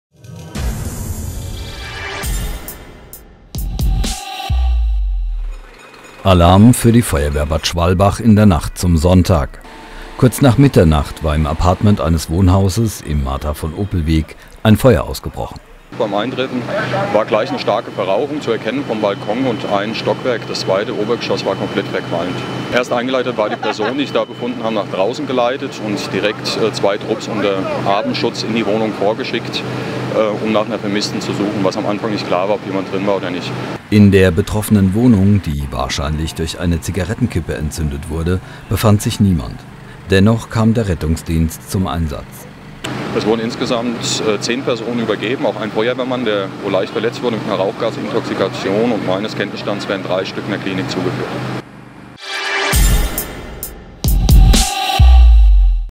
Brand BadSchwalbach
Eine Zigarettenkippe war wahrscheinlich der Auslöser für einen Brand in einem Mehrfamilienhaus.